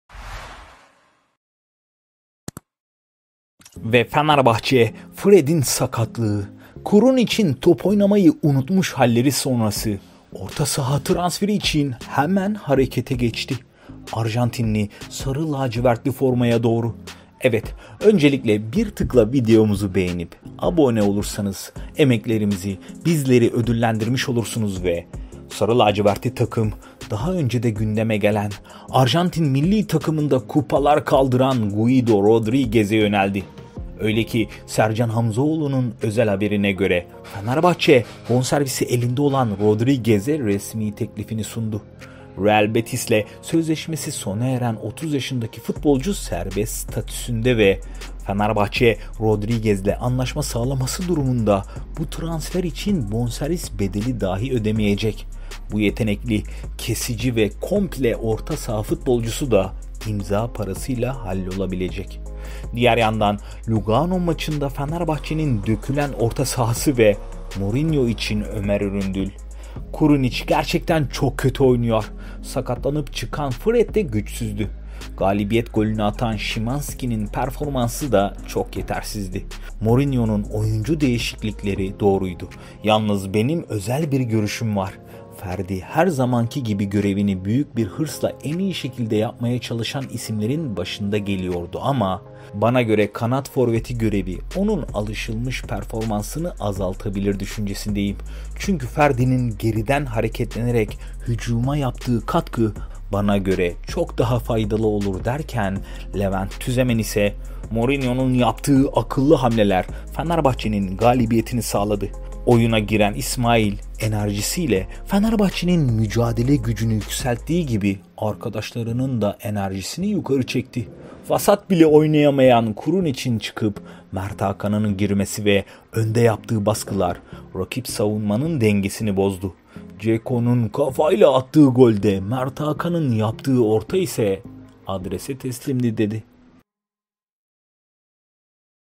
SON DAKİKA! Yılın Bombası Patladı; Bedavaya Fenerbahçe'de..
Fenerbahçe, Fred'in sakatlığı krunicin top oynamayı unutmuş halleri sonrası orta saha transferi için hemen harekete geçti.

Sarı-lacivertli takım, daha önce de gündeme gelen Arjantin milli takımında kupalar kaldıran orta saha Guido Rodriguez'e yöneldi. Sercan Hamzaoğlu'nun haberine göre, Fenerbahçe, bonservisi elinde olan Rodriguez'e resmi teklif sundu.

Real Betis ile sözleşmesi sona eren 30 yaşındaki futbolcu serbest statüde ve Fenerbahçe, Rodriguez ile anlaşma sağlaması durumunda bu transfer için bonservis bedeli ödemeyecek. bu yetenekli kesici ve komple orta alan futbolcusu da imza parasıyla hallolabilecek

diğer yandan lugano maçında fenerbahçenin  dökülen orta sahası ve mourinoh için
 Ömer Üründül   "Krunic gerçekten çok kötü oynuyor, sakatlanıp çıkan Fred de güçsüzdü. Galibiyet golünü atan Szymanski'nin performansı da çok yetersizdi. Mourinho'nun oyuncu değişiklikleri doğruydu. Yalnız benim özel bir görüşüm var. Ferdi her zamanki gibi görevini büyük bir hırsla en iyi şekilde yapmaya çalışan isimlerin başında geliyordu ama bana göre kanat forveti görevi onun alışılmış performansını azaltabilir düşüncesindeyim. Çünkü Ferdi'nin geriden hareketlenerek hücuma yaptığı katkı bana göre çok daha faydalı olur."

Levent Tüzemen: "Mourinho'nun yaptığı akıllı hamleler, Fenerbahçe'nin galibiyetini sağladı. oyuna giren İsmail enerjisiyle Fenerbahçe'nin mücadele gücünü yükselttiği gibi arkadaşlarının da enerjisini yukarı çekti. Vasat bile oynayamayan Krunic'in çıkıp Mert Hakan'ın girmesi ve önde yaptığı baskılar, rakip savunmanın dengesini bozdu. Dzeko'nun kafayla attığı golde Mert Hakan'ın yaptığı orta adrese teslimdi."